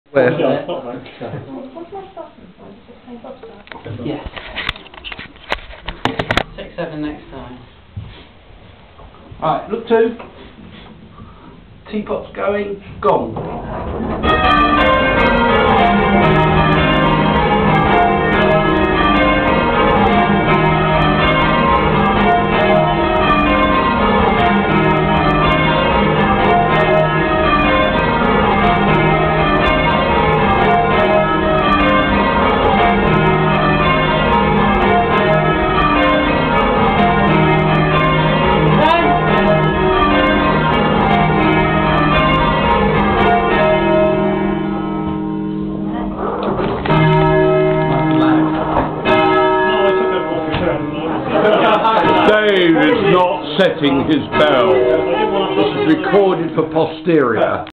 Rounds at Thorncombe Dorset
Ringing before a quarter peal .